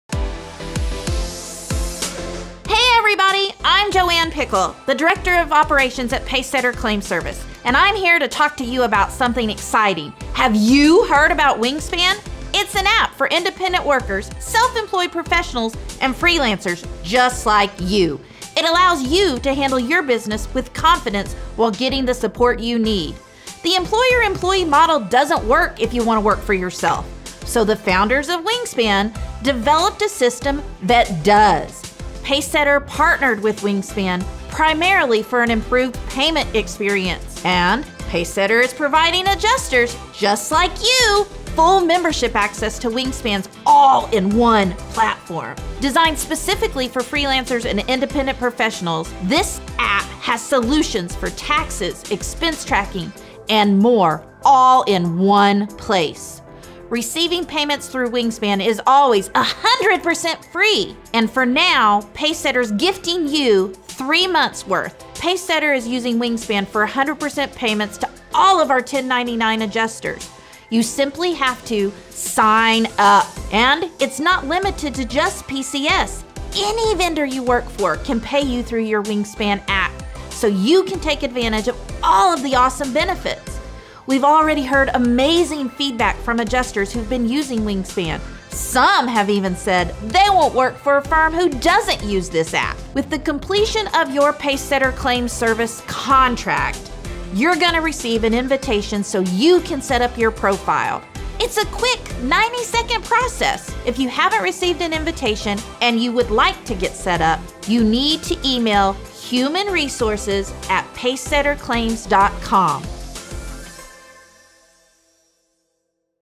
Introducing Wingspan | App for Independent Workers, Self Employed Professionals and Freelancers
Pacesetter is providing adjusters full membership to Wingspan's All-in-One Platform.